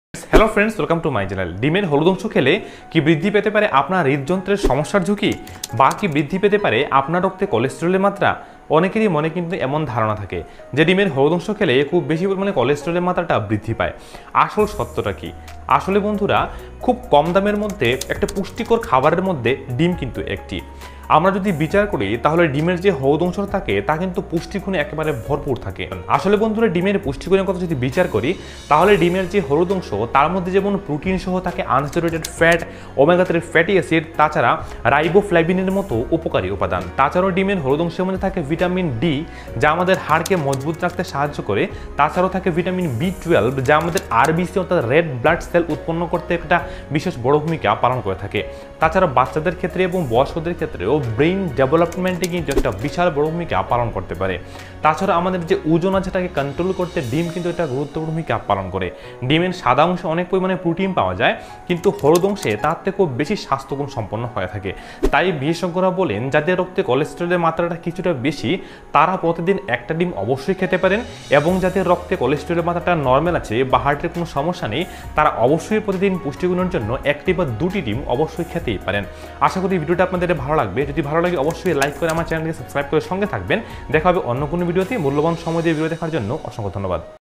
ডিমের সাদা অংশ খাবেন না হলুদ অংশ কোনটা উপকারী | কোলেস্টেরল হার্টের সমস্যায় কি ডিম খাওয়া যাবে ।
ডিমের সাদা অংশ খাবেন না হলুদ অংশ কোনটা উপকারী
কোলেস্টেরল হার্টের সমস্যায় কি ডিম খাওয়া যাবে

Health benefits of egg yolk
Egg health benefits
কোলেস্টেরল এবং হার্টের সমস্যায় ডিমের সাদা অংশ ভালো না হলুদ অংশ

India(North East)
Tripura (Agartala)